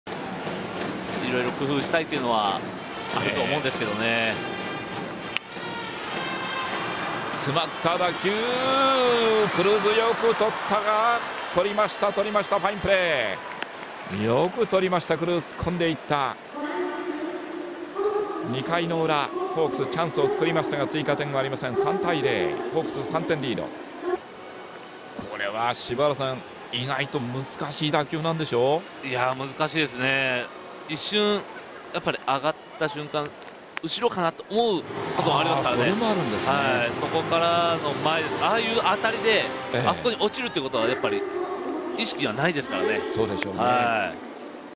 【プロ野球パ】どんづまりのフライが上がる… 落ちると思ったその瞬間クルーズが現れる！ 2015/03/18 H-M
2回、ソフトバンクの今宮が放ったどんづまりの内野フライをクルーズがスライディングキャッチ。落ちれば得点につながる可能性もあったが、チームを救う大きなプレーとなった。 （3月18日 ソフトバンク-ロッテ）